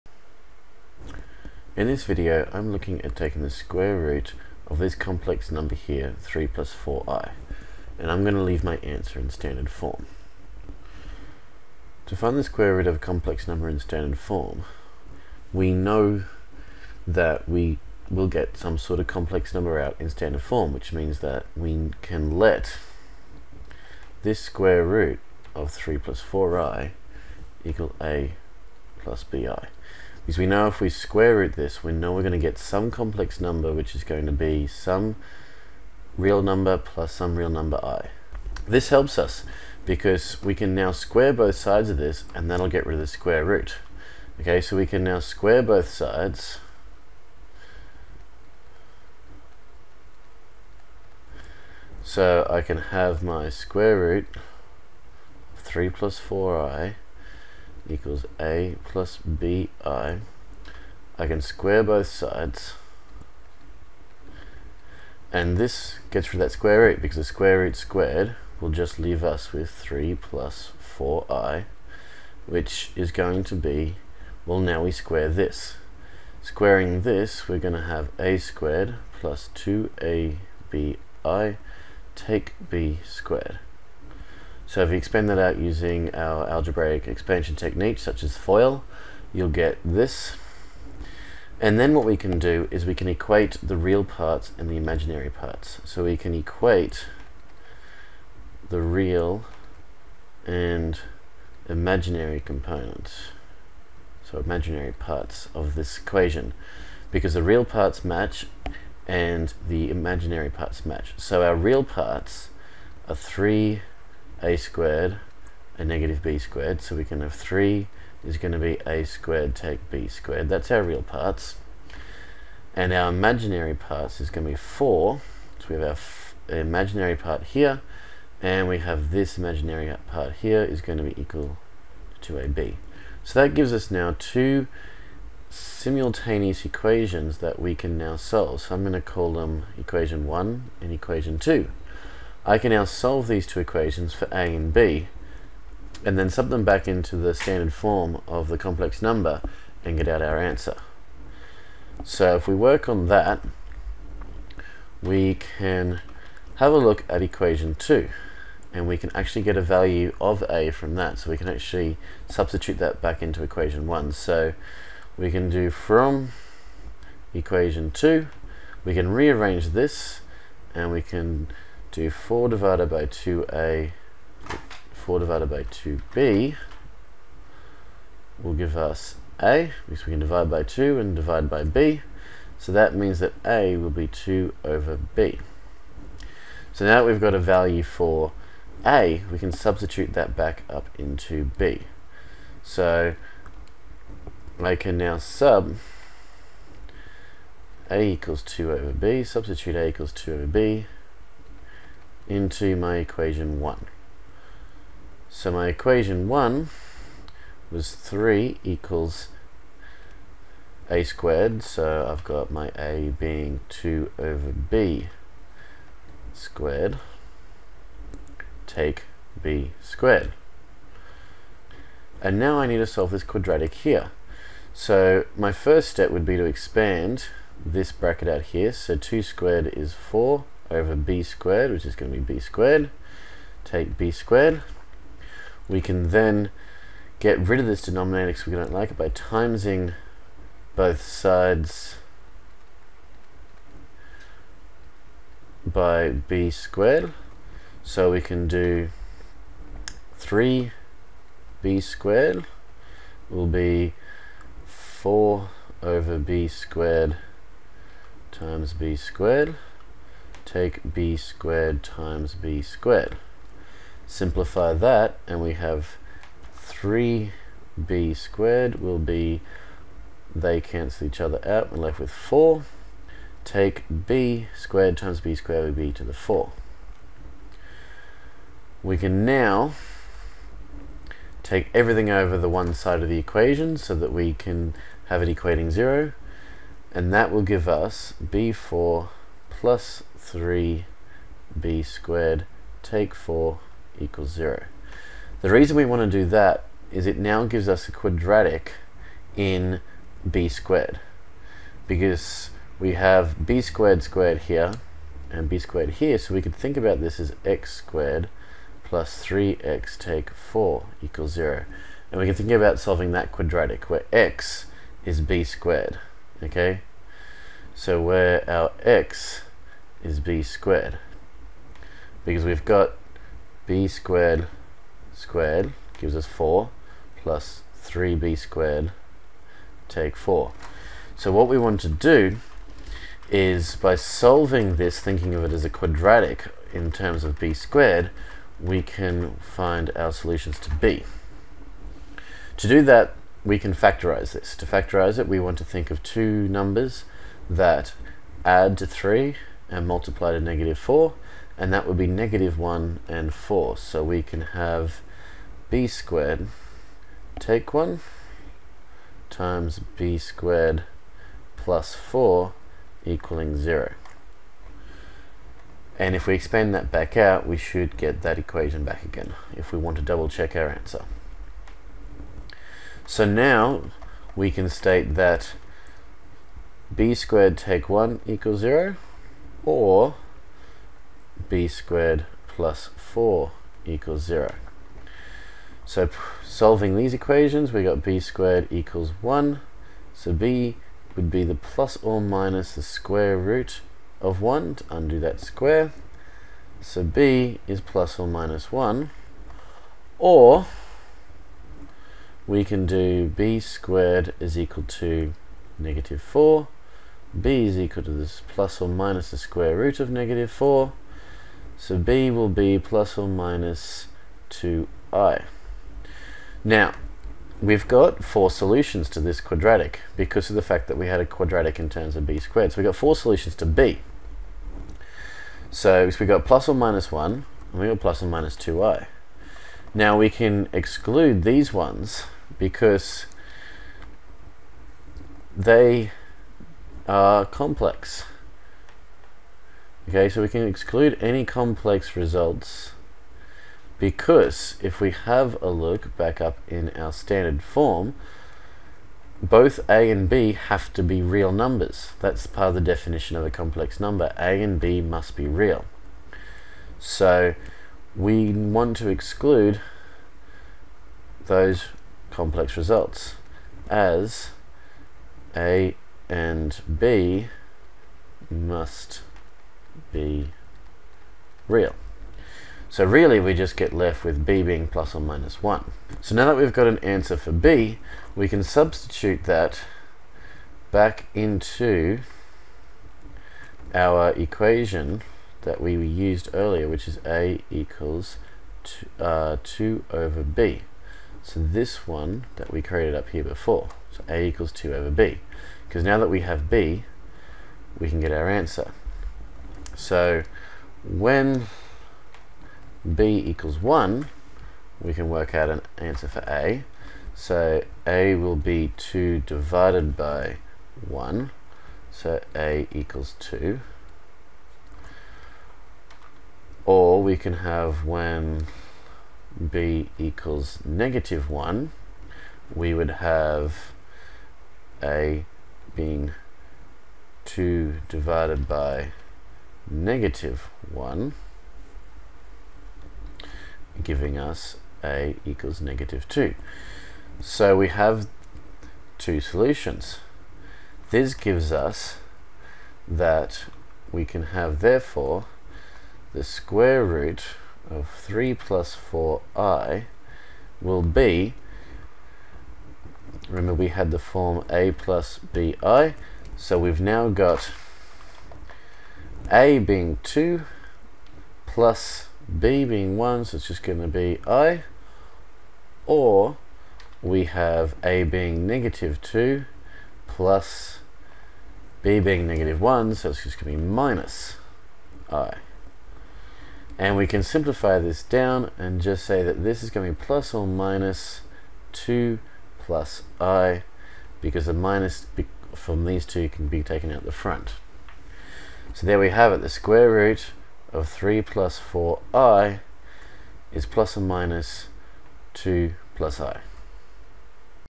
Square Root of Complex Numbers
This video shows an example of calculating the square root of a complex number in standard form (a+bi)